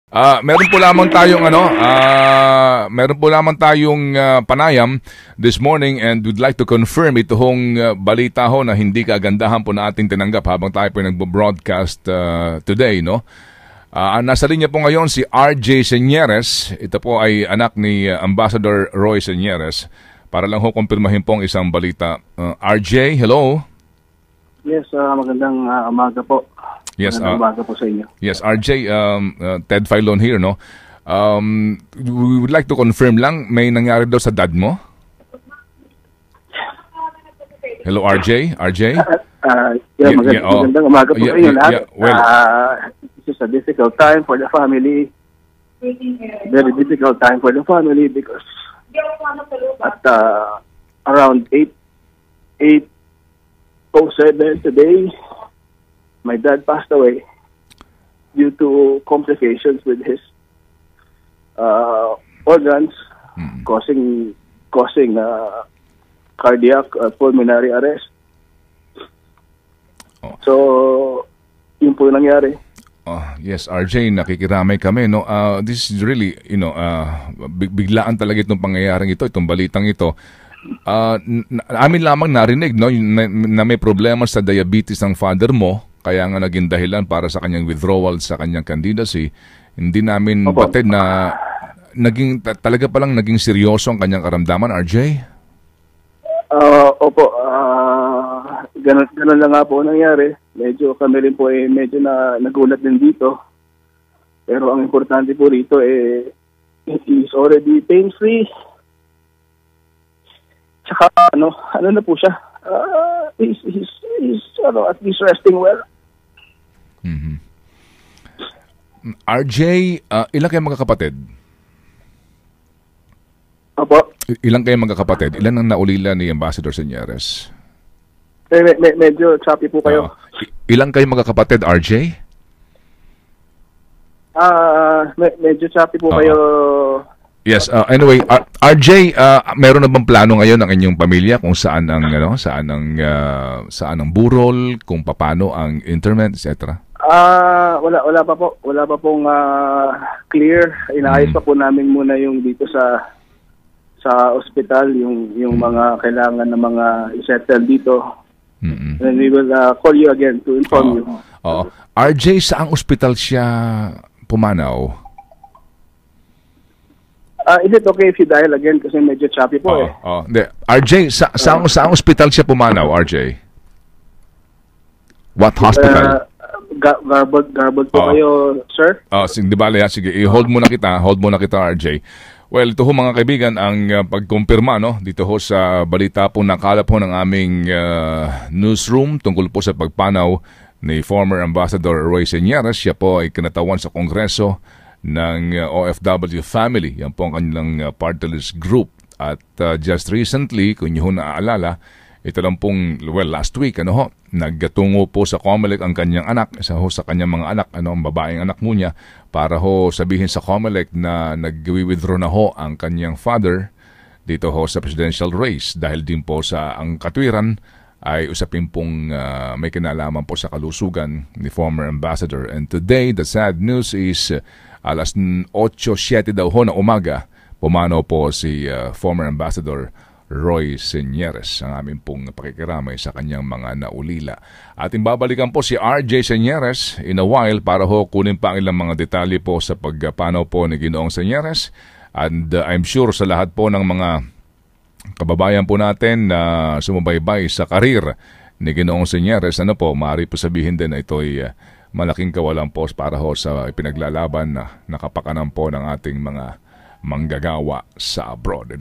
Seneres dies of cardiac arrest
Days after he backed out of the 2016 presidential race, OFW Family party-list Representative Roy Señeres Sr. passed away early Monday.